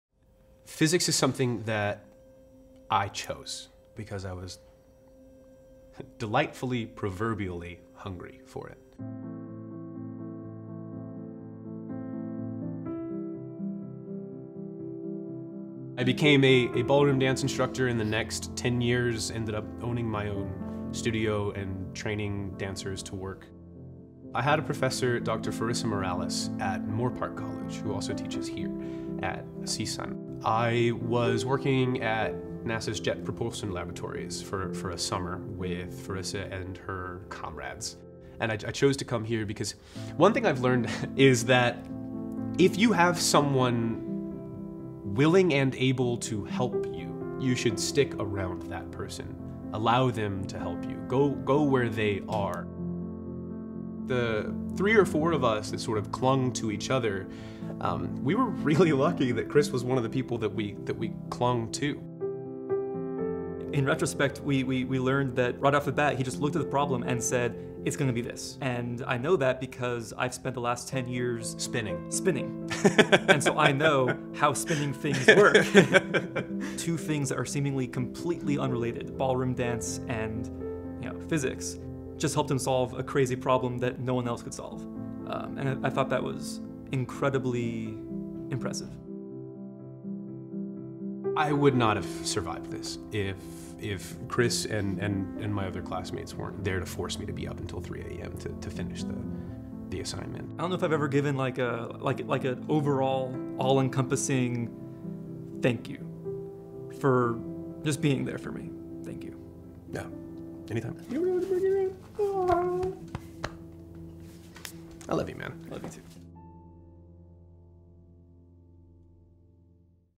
Justin Bracks '19 - B.S. in Astrophysics
Justin Bracks, 31 of Pasadena, will be spending the next few years building a satellite balloon that will launch several million dollars-worth of equipment into space from a research station in Antarctica in an effort to get an better understanding of our universe.